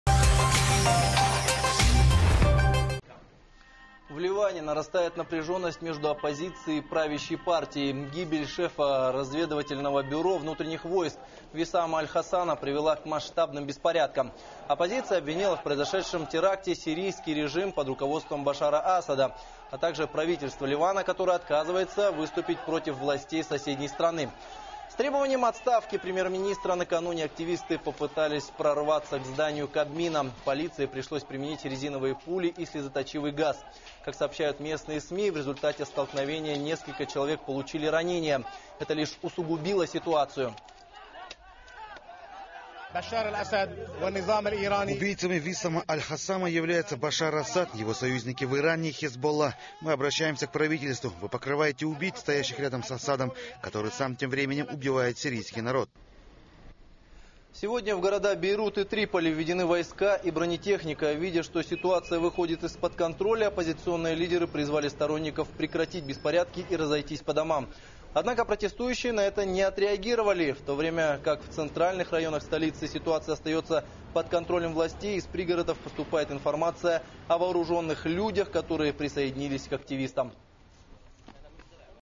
В Бейрут и Триполи введены войска
В Ливане нарастает напряженность между оппозицией и правящей партией. Гибель шефа разведывательного бюро внутренних войск Висама аль-Хасана привела к масштабным беспорядкам. Оппозиция обвинила в произошедшем теракте сирийский режим под руководством Башара Асада, а также Правительство Ливана, которое отказывается выступить против властей соседней страны.